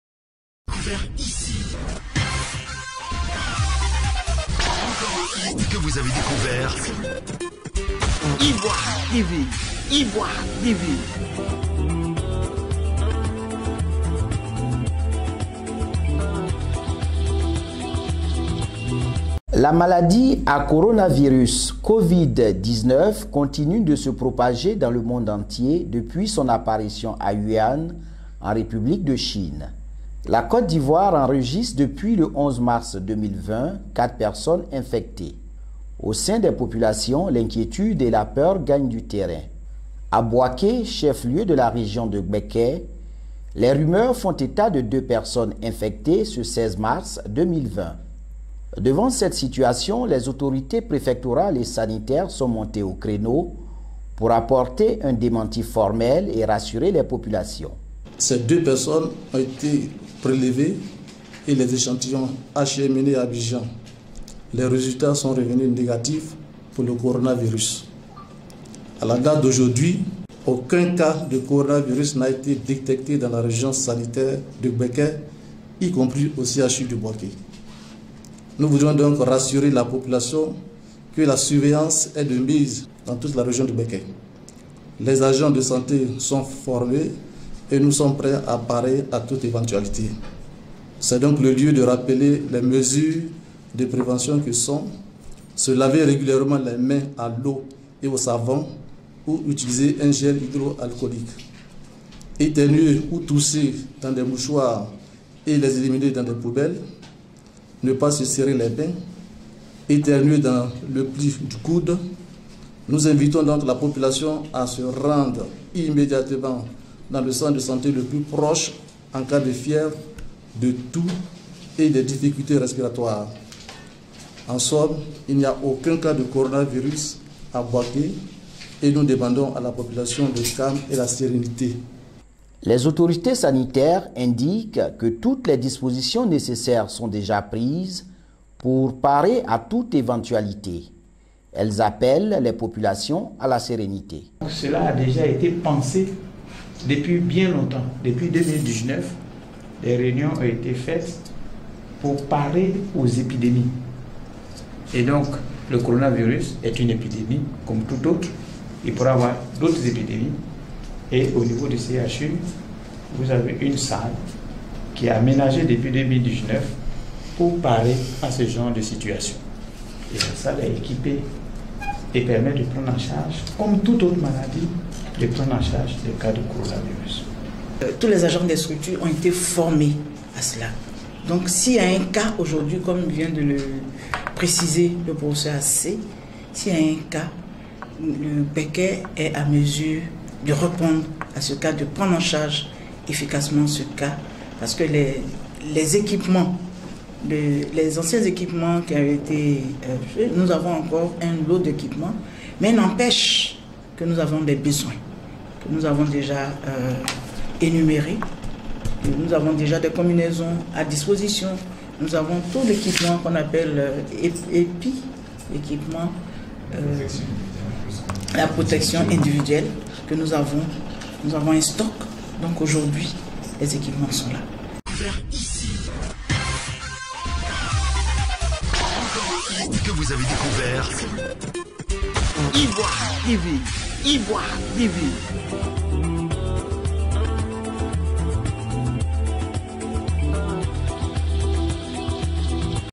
Bouaké/ Propagation du Coronavirus : les autorités rassurent
Les autorités de Bouaké, Chef-lieu de la région de Gbêkê ont démenti cet après midi la rumeur selon laquelle, Bouaké enregistrerait des cas suspects d'infectés de la maladie à  coronavirus . Ces patients seraient internés au Centre Hospitalier Universitaire (CHU).
Cette rumeur est stoppée nette par les autorités préfectorales et Sanitaires de la ville.
 Le Secrétaire Général 1, ANKOUN Kouadjo, assisté par   des experts des questions  de santé, a rassuré les populations. La Directrice Régionale de la Santé et de l'hygiène publique, Dr BAMBA et Pr. ASSE Vincent,  ont indiqué avoir le matériel nécessaire pour parer à toutes éventualités.

Propos recueillis par François M'BRA II & Fabrice KRA